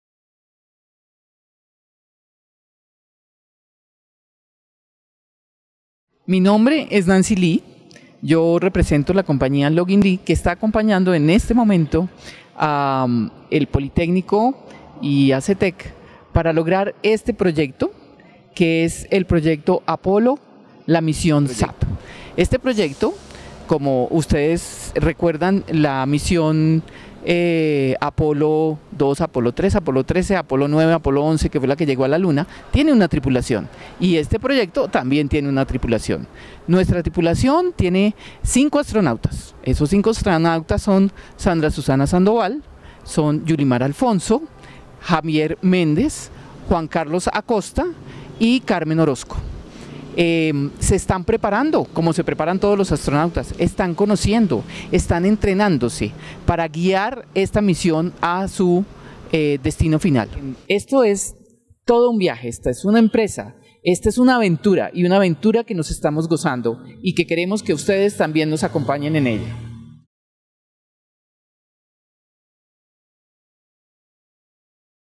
Representante de Login Lee da la bienvenida a la tripulación del proyecto SAP
El área de Talento Humano del Poli contrató a la firma Login Lee, para apoyar a nuestra Institución con el proceso de gestión de cambio para el nuevo proyecto SAP. El objetivo es acompañar a todas las personas que hacen parte del equipo, durante la transición entre el antiguo sistema y el nuevo. La Facilitadora del proceso, Nancy Lee, le dio la bienvenida a toda tripulación.